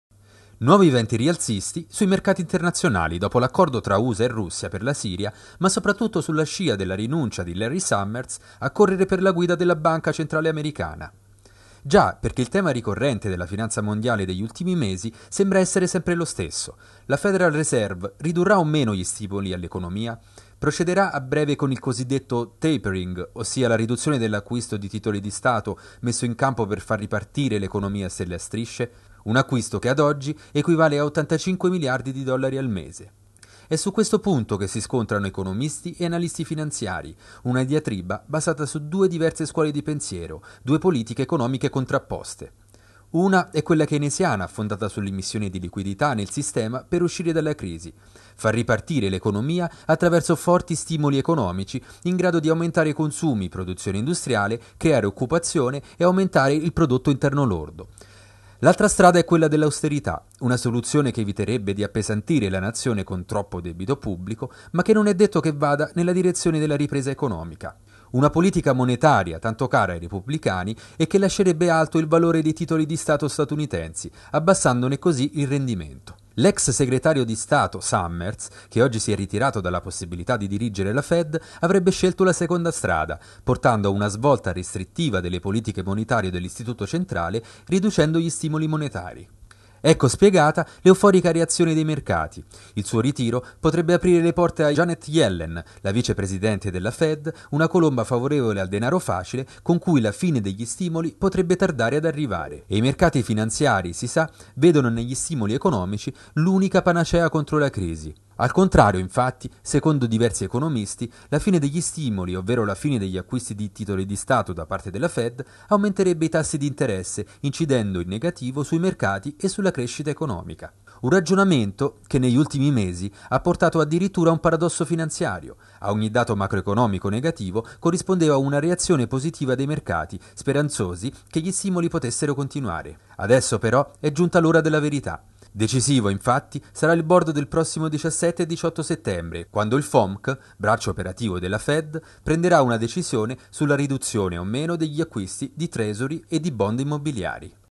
Fed verso il nuovo vertice, quali saranno le prossime politiche monetarie?
Larry Summers si ritira dalla carica di governatore della Fed. Quali saranno le prossime decisioni della banca centrale americana su politiche monetarie e quanto incideranno sull'economia reale.